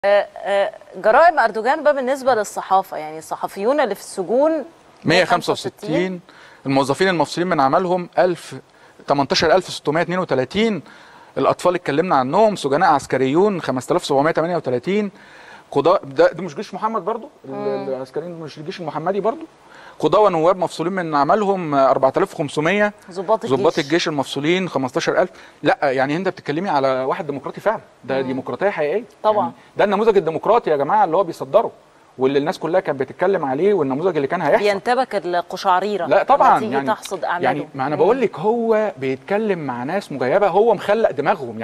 الآن | إكسترا نيوز تكشف حقيقة أردوغان.. جرائم الديكتاتور التركي ضد قضاة ونواب وصحفيين
الآن | إكسترا نيوز تكشف حقيقة أردوغان.. جرائم الديكتاتور التركي ضد قضاة ونواب وصحفيون     | 10  أكتوبر 2019